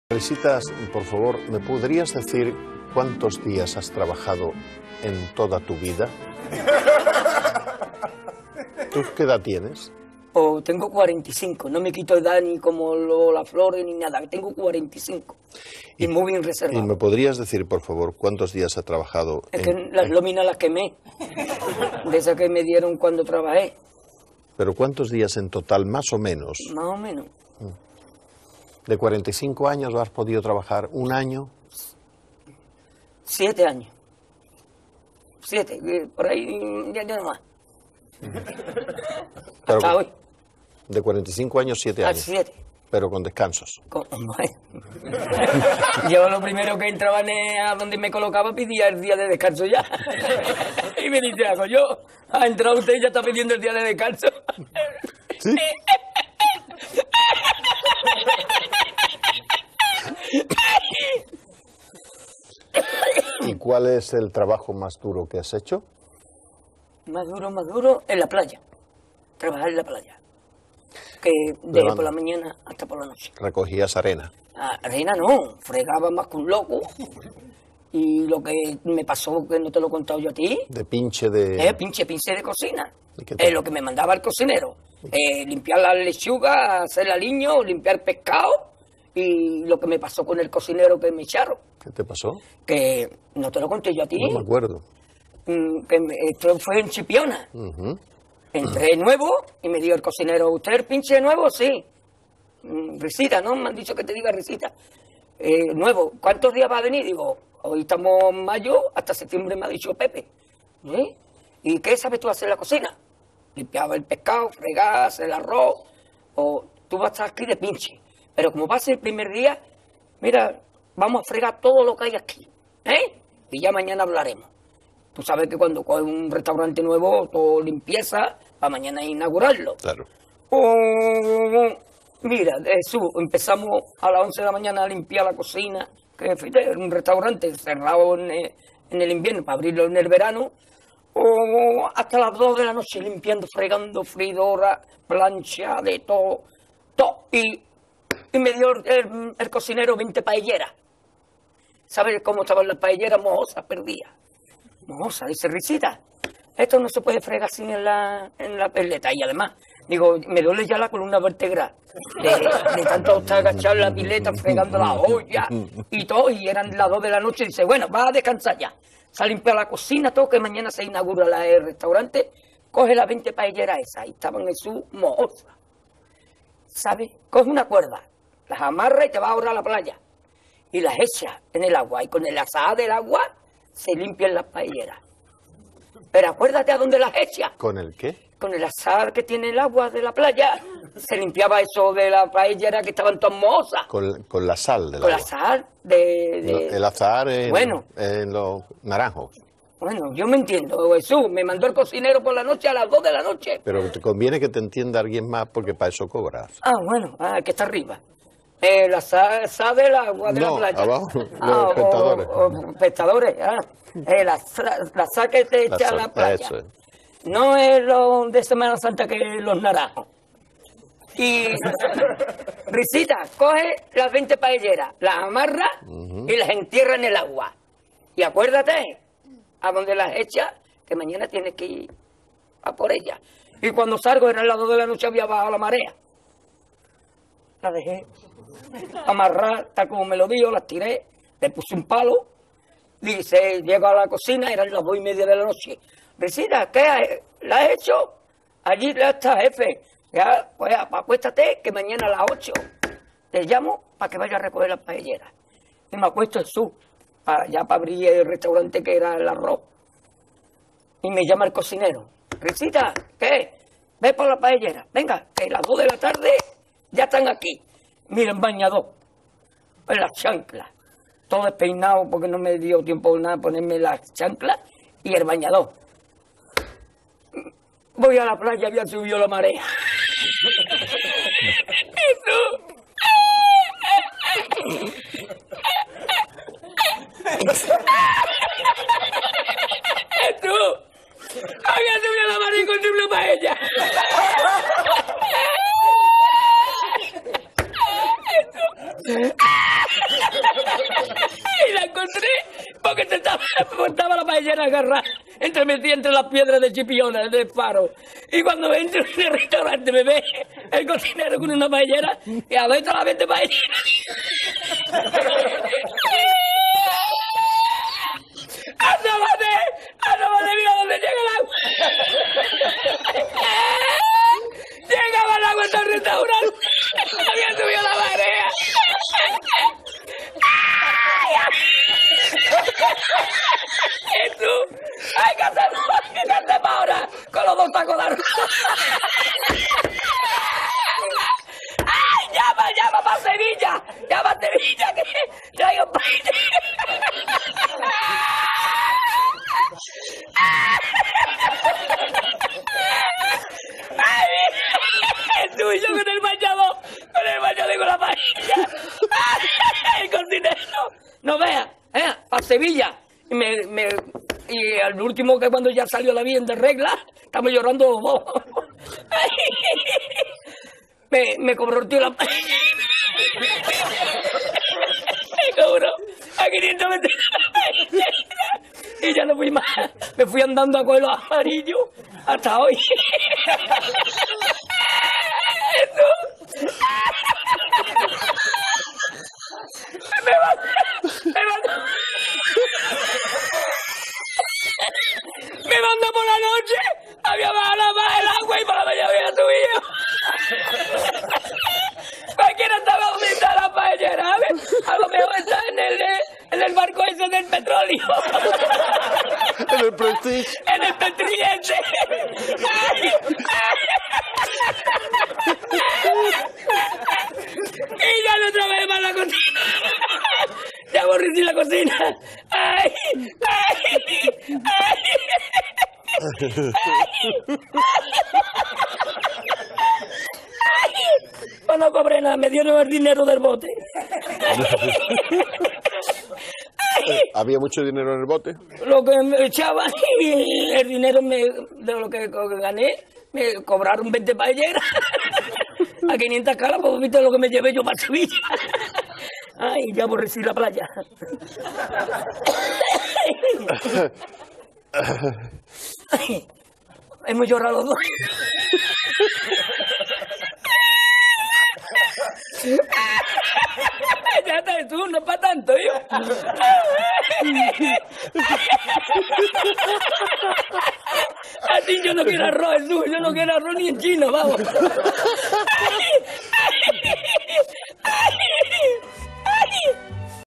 Risitas alias Francesco Zombori magyar felirattal
Risitas, más néven Francesco Zombori először igazán magyarul: semmi MacBook, semmi román-magyar, csak a paella és az a felejthetetlen röhögés! Az ikon életéről és munkásságáról a KultúrTapas Spanyol Magazinban

Magyarázatok a szöveghez:

Lola Flores (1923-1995): híres andalúz flamencoénekesnő.
Chipiona: andalúz kisváros Cádiz tartományban, az Atlanti-óceán partján
„la sal" helyett „el az[ah]ar"-t mond, vagyis só helyett narancsvirágot. Az „l" és az „r" hangokat keveri, így
nem veszi észre a különbséget
A műsorvezetőt Jesús Quinterónak hívják.
Virgen de Regla: Chipiona védőszentje, ünnepét szeptember 8-án tartják.
Los Amarillos: andalúz busztársaság, nevét a buszok sárga színéről kapta.
Prestige: olajszállító hajó, a galíciai partoknál süllyedt el.